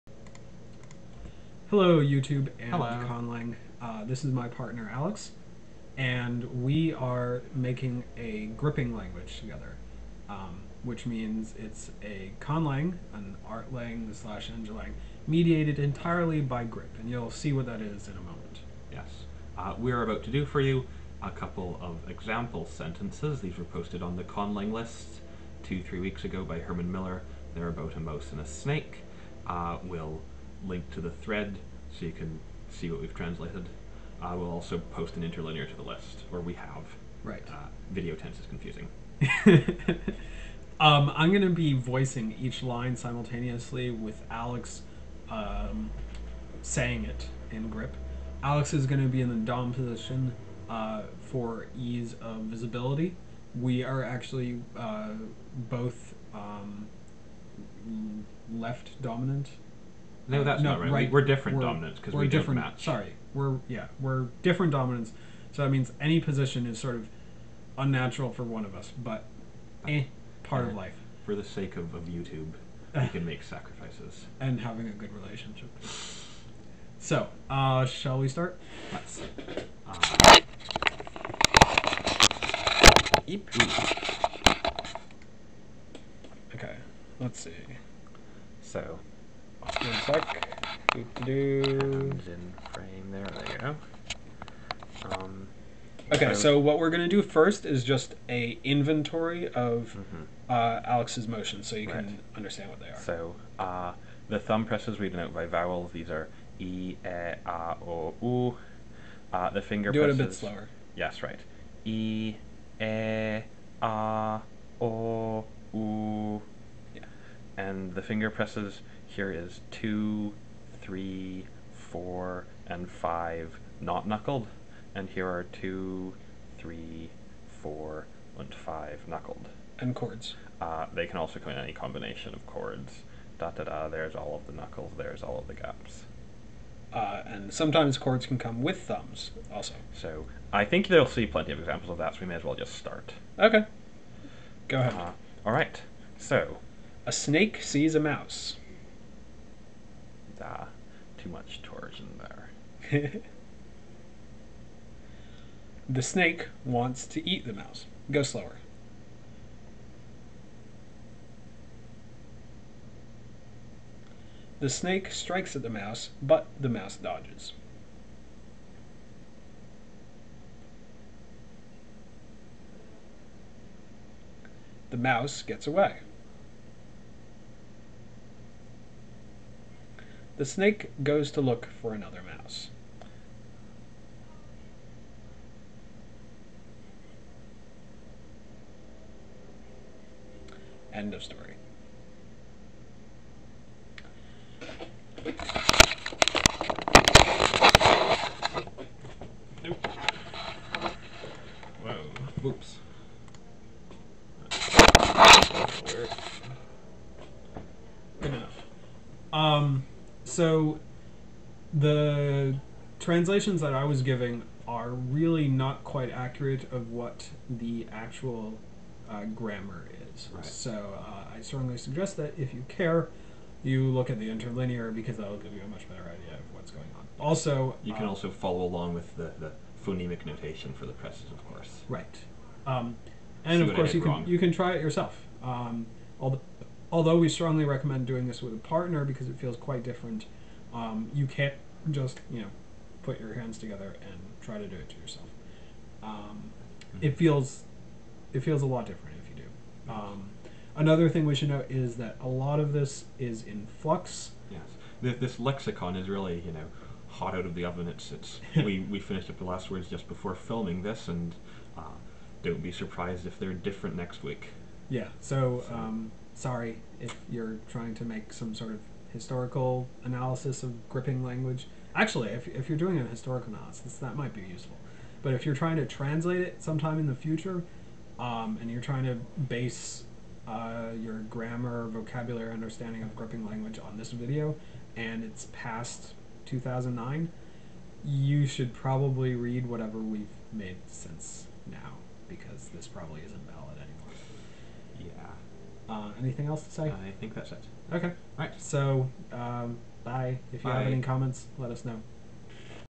Gripping language snake and mouse story translation
Transcription in red notation: 3a23a e 5'45e 35a24a a 5'35; e 45'i 24o 25a25i; è 25i25i 24o 25'25o e; e 25'a25ó e; a 25'a25ó 35'o 3a23a 35'e; e e a a